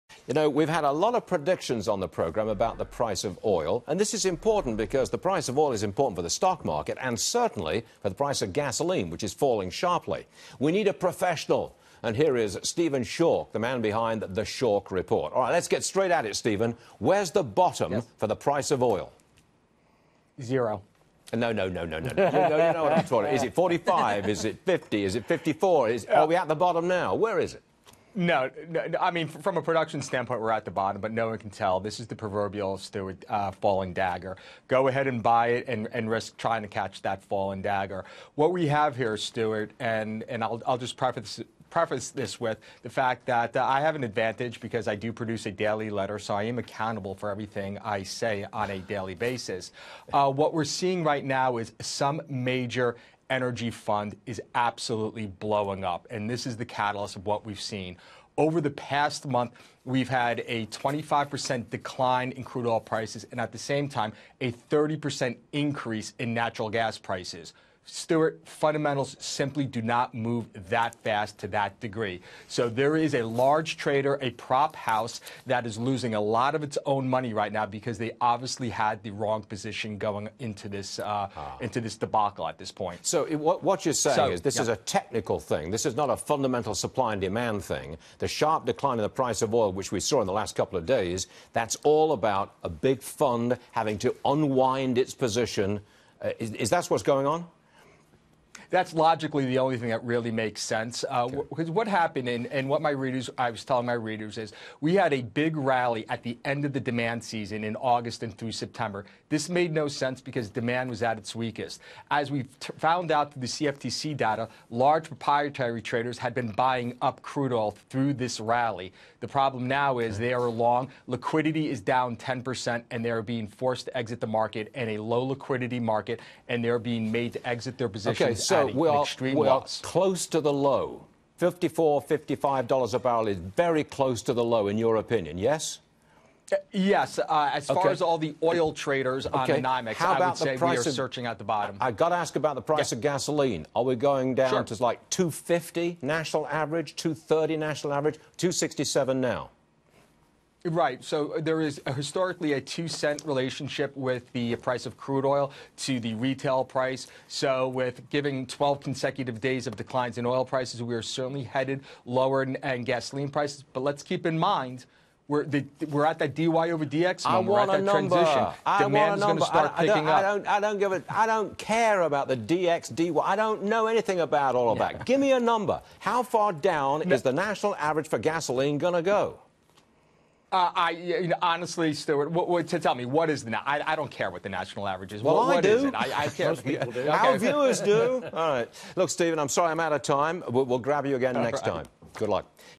Where is the bottom for oil prices?
The Schork Report Principal Stephen Schork on the outlook for oil prices.